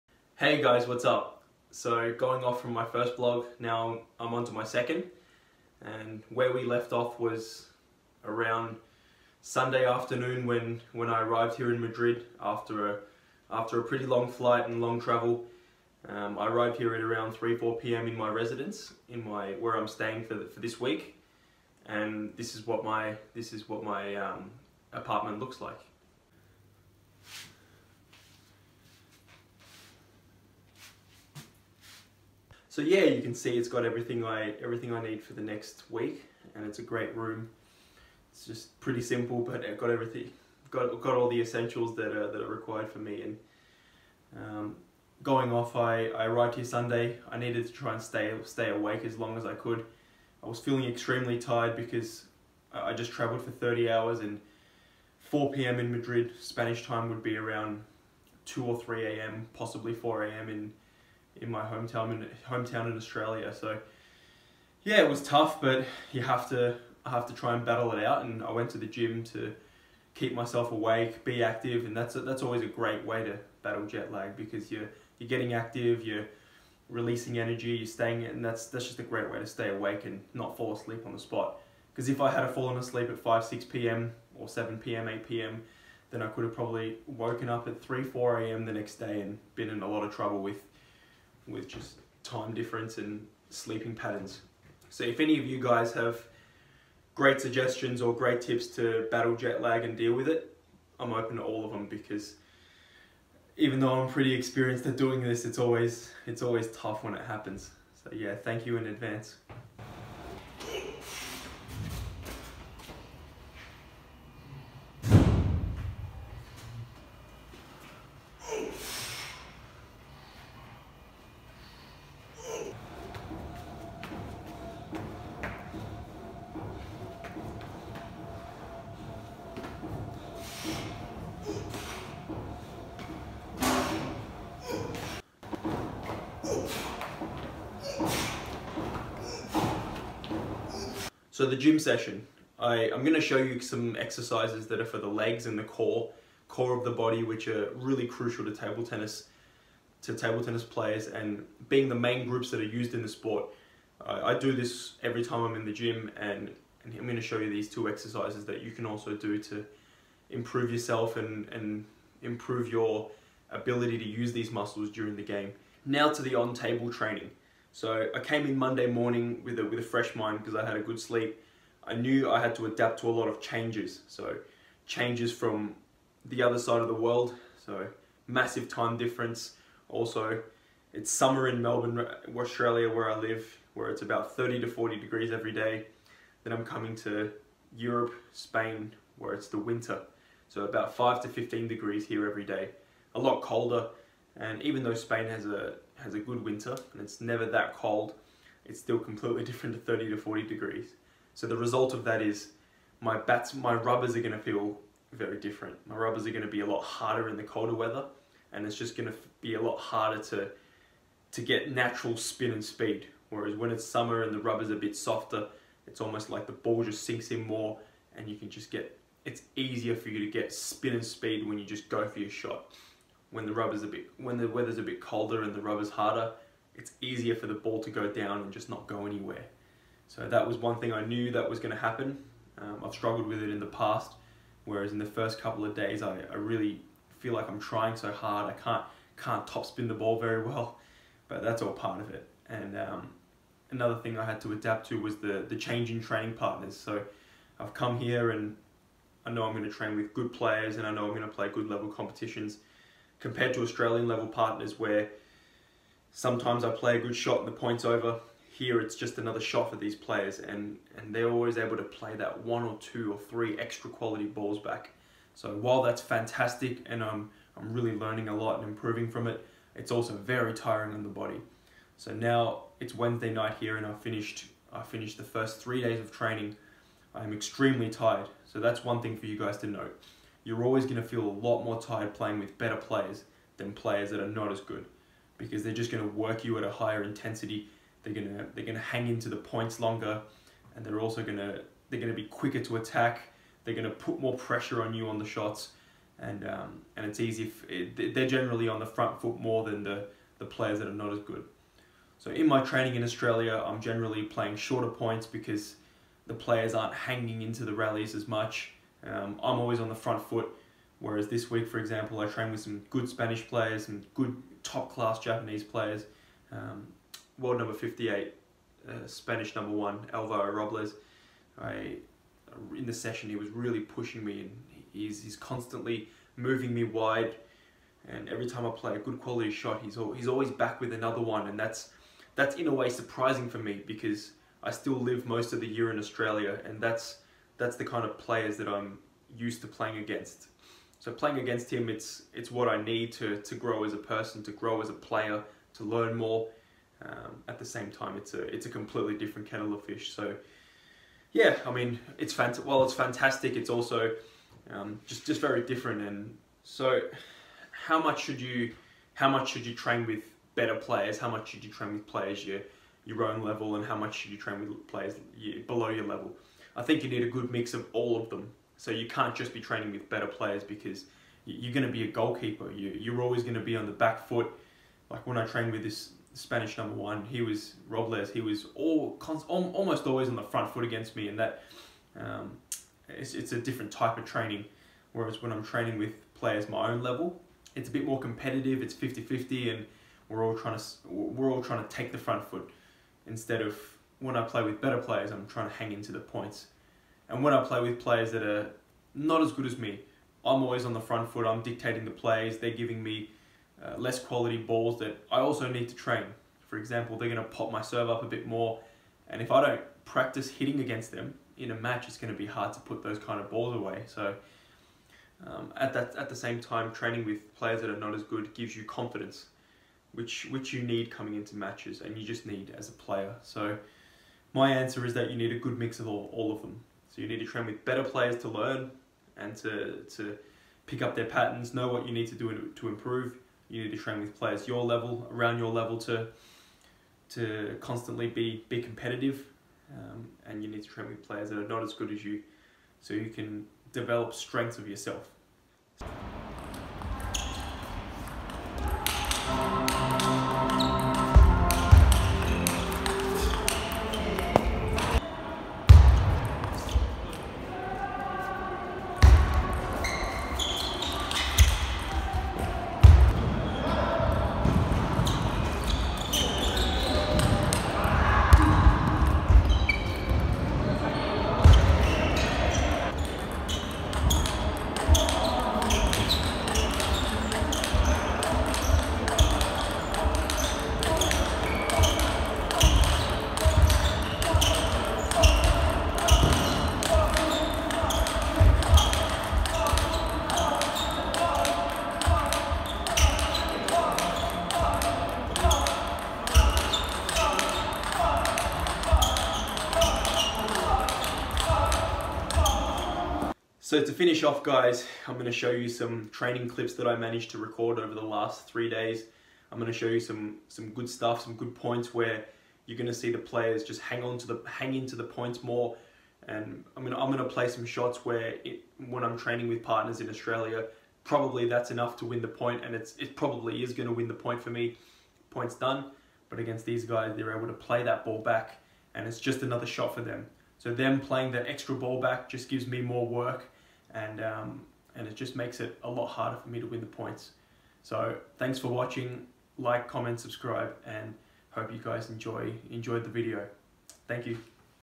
VLOG #2. FIRST DAYS TRAINING IN MADRID
The training is amazing here in Madrid, Spain. I have been playing with some top class players including Japanese, Koreans, Spanish National team and World Number 58 Alvaro Robles
Here is what some of it looks like day to day!